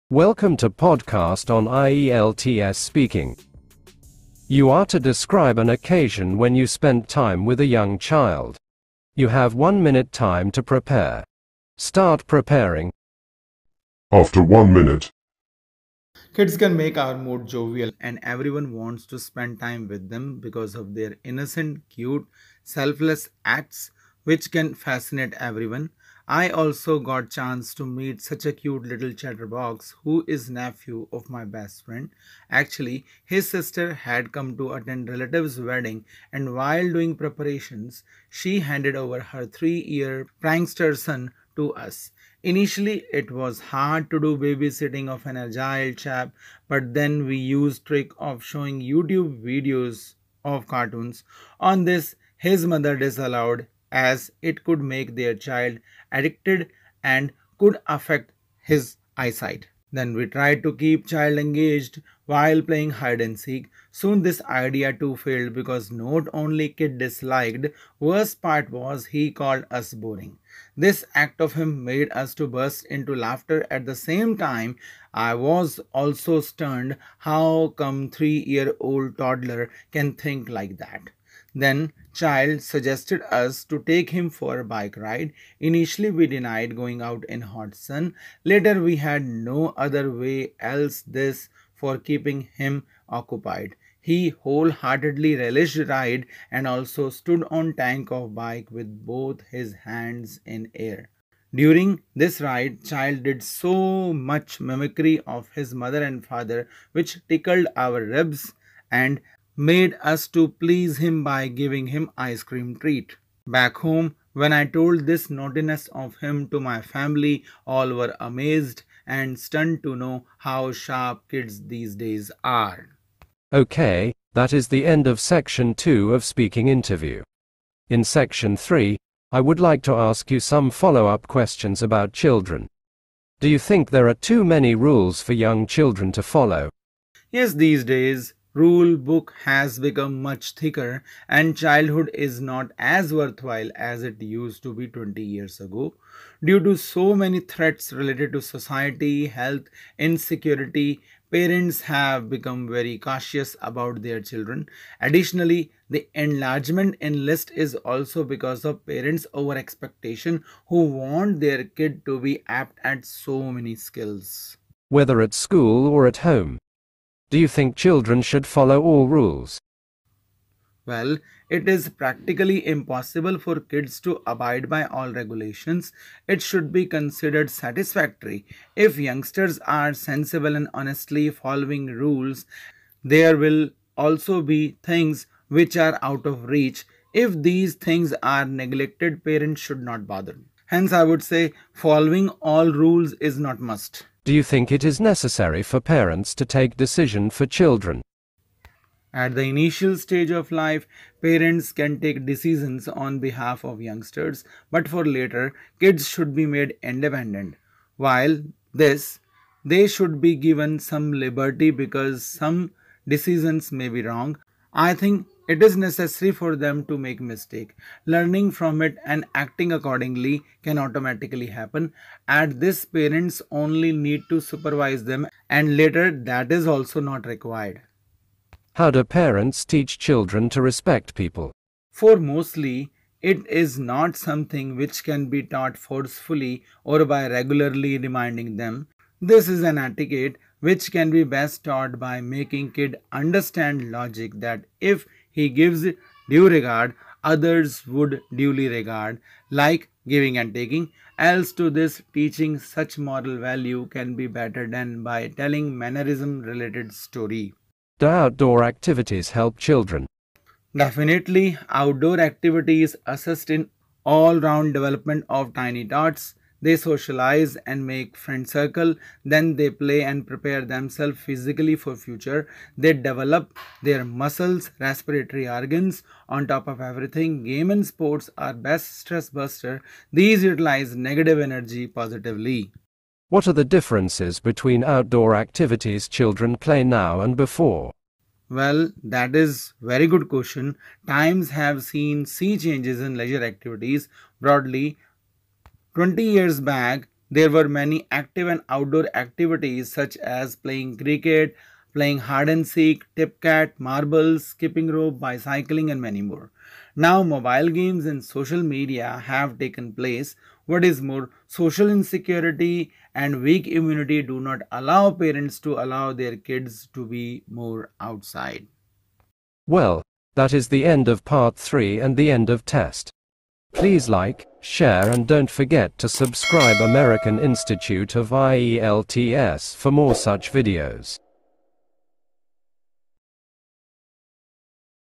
Describe an occasion when you spent time with a young child | 8 Band SAMPLE | IELTS SPEAKING
9 BAND SAMPLE | IDEAS AND VOCABULARY